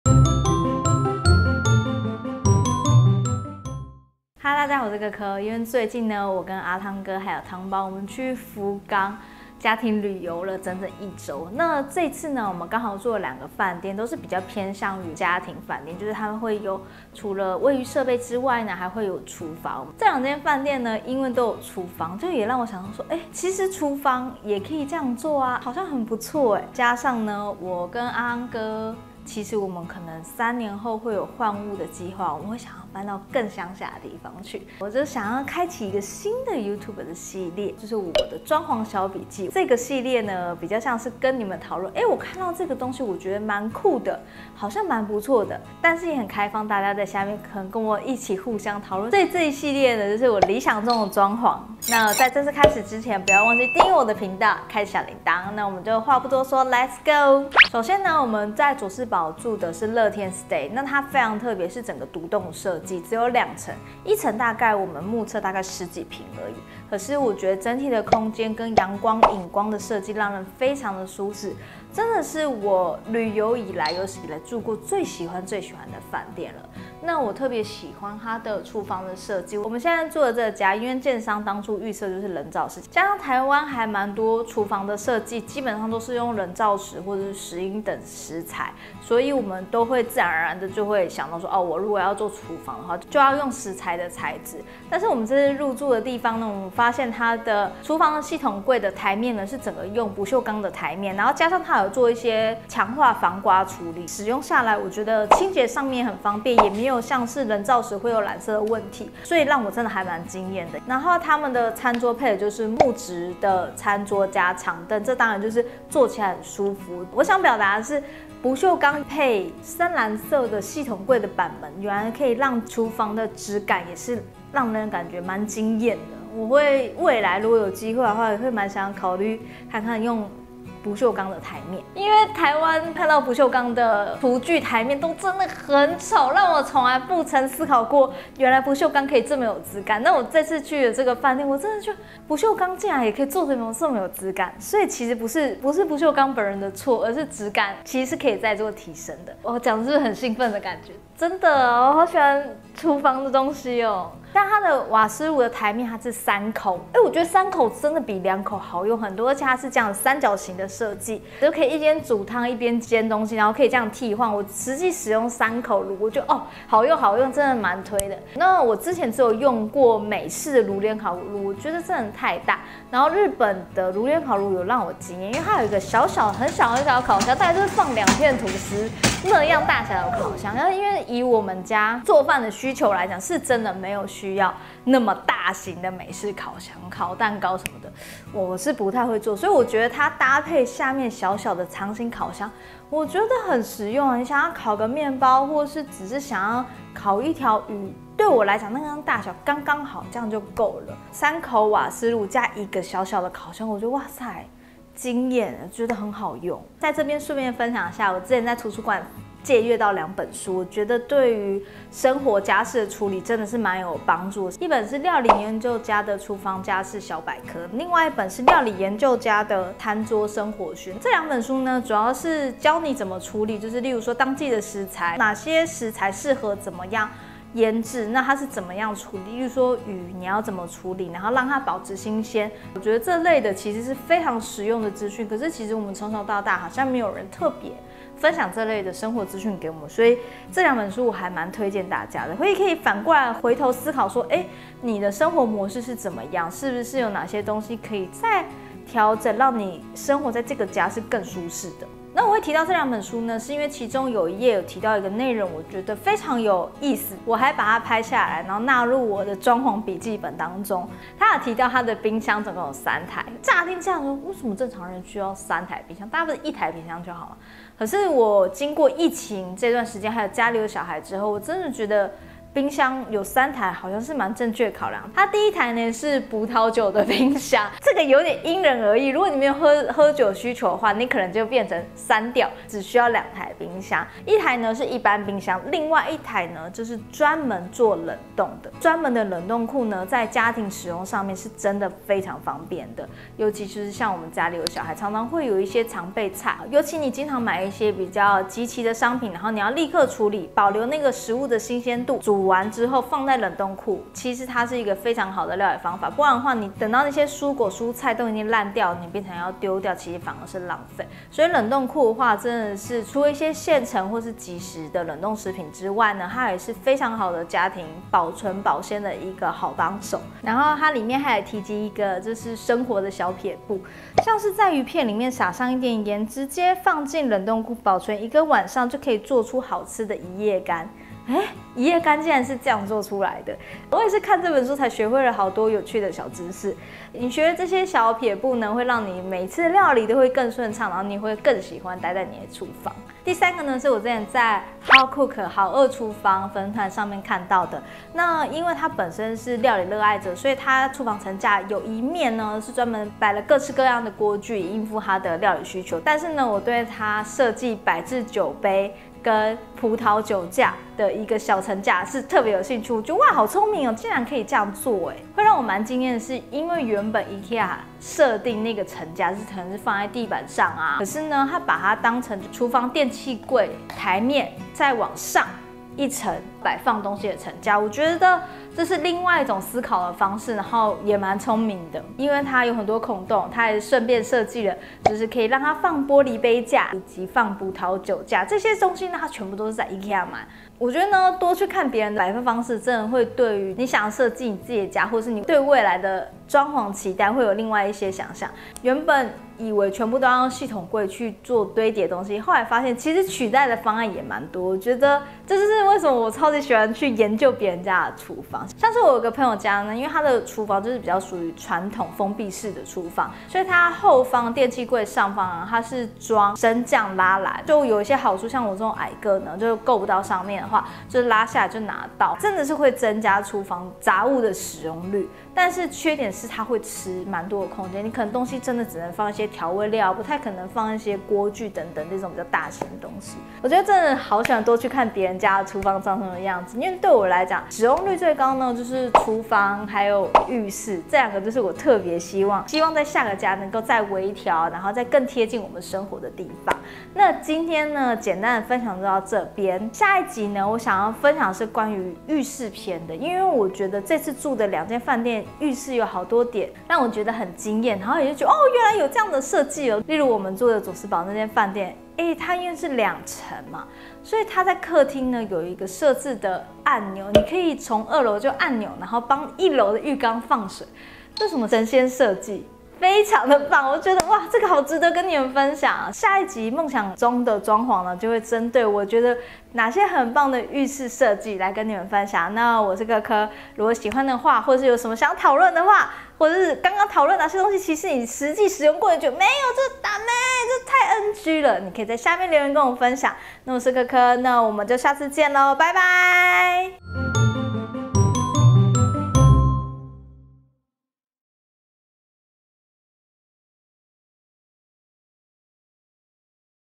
理想中的裝潢 01 - 廚房篇｜日式爐連烤、不鏽鋼檯面超實用！
00:00 開場
00:57 佐世保 | 樂天 STAY HOUSE
01:20 廚具檯面設計
02:34 瓦斯爐設計
02:47 爐連烤爐設計
03:26 我喜歡的廚房
06:49 結尾